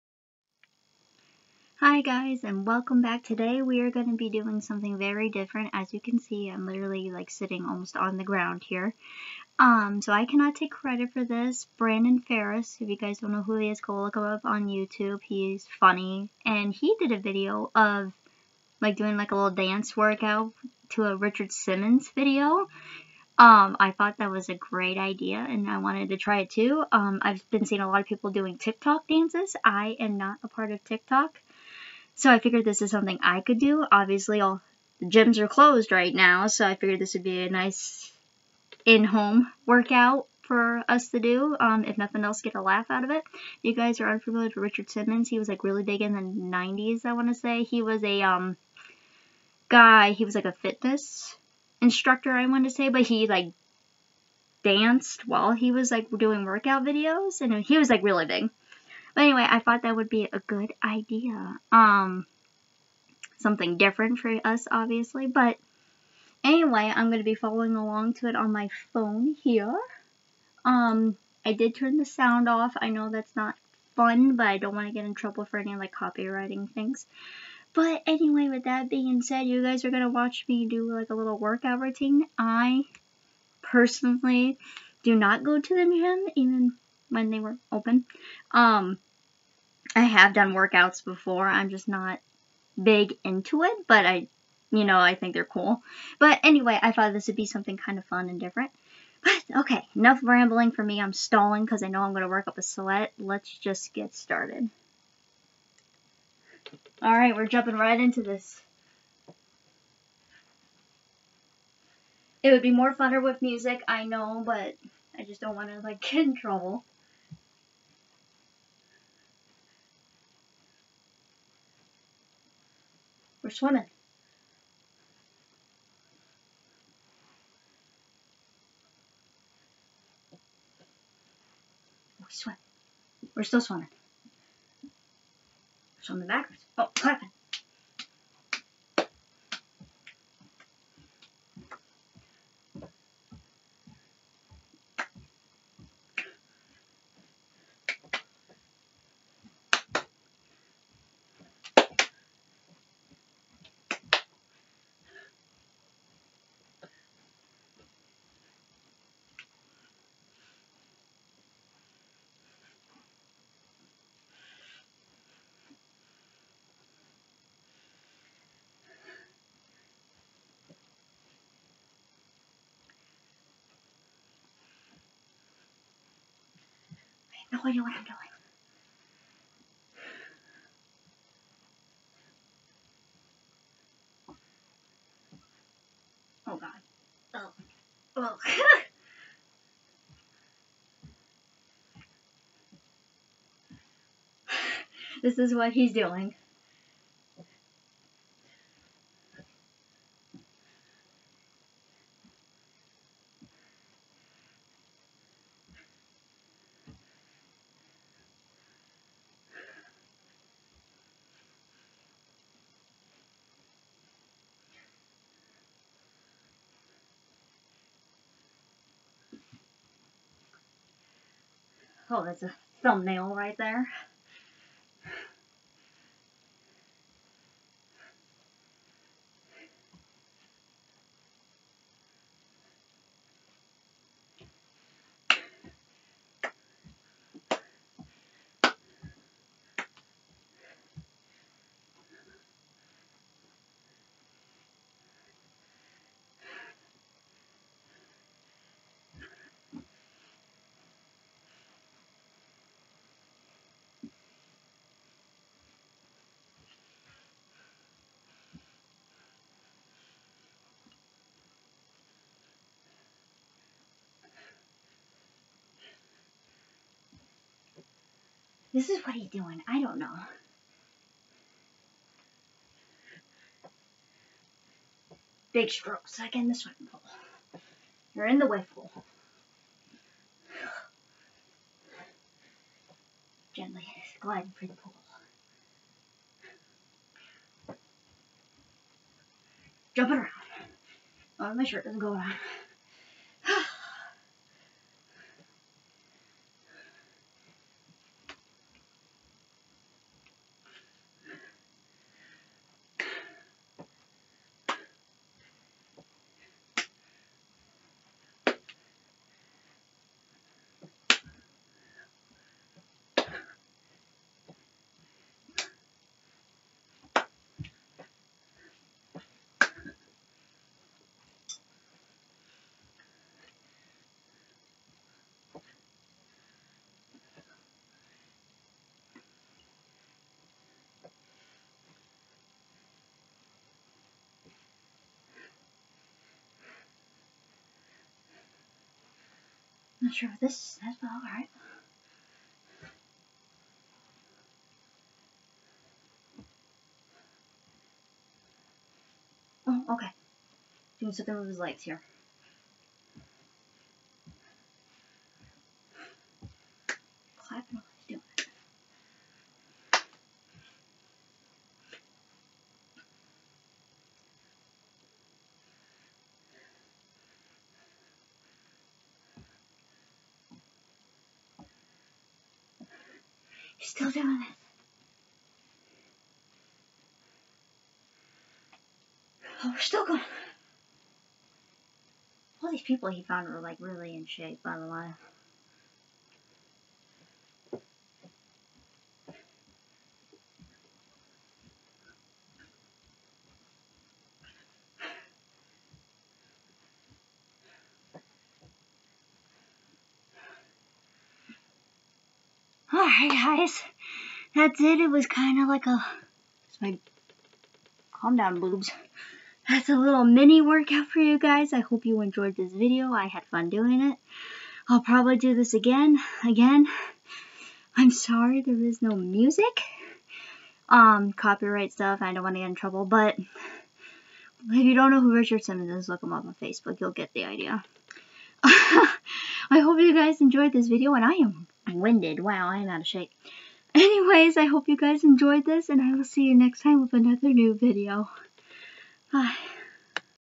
I did a mini workout to a Richard Simmons dance video!
come do a mini workout with me to a Richard Simmons video and lets have some fun! Not my original idea.